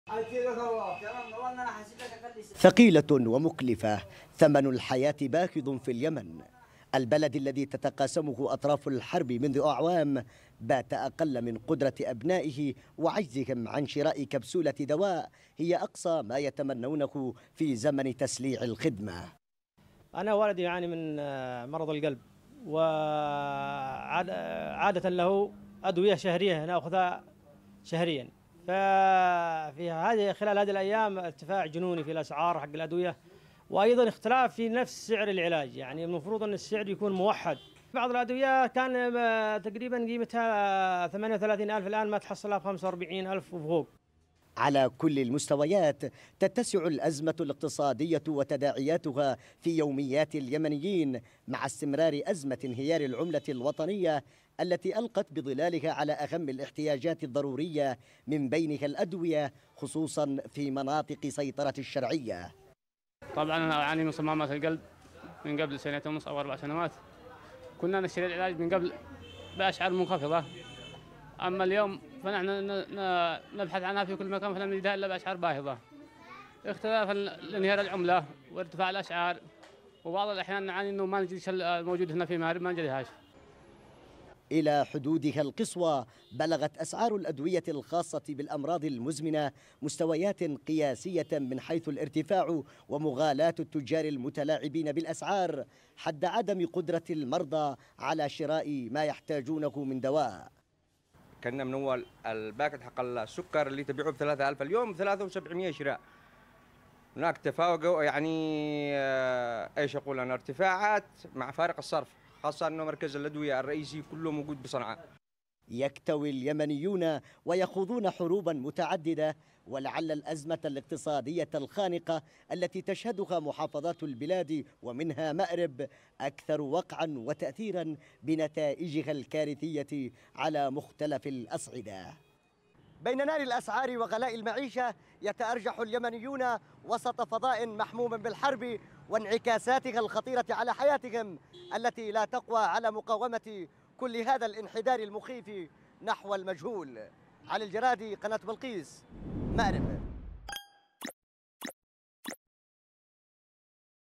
ارتفاع أسعار السلع الأساسية في محافظة مأرب جراء انهيار العملة | تقرير:علي الجرادي
مع انهيار العملة الوطنية أمام العملات الأجنبية تشهد السوق المحلية في محافظة مأرب ارتفاعا جنونيا في أسعار السلع المختلفة ومنها الأدوية.
حيث يشكو المواطنون  من ارتفاع أسعار الأدوية إلى ضعف ما كانت عليه خلال الفترة الماضية، إضافة إلى انعدام بعض الأدوية الخاصة بالإمراض المزمنة. ..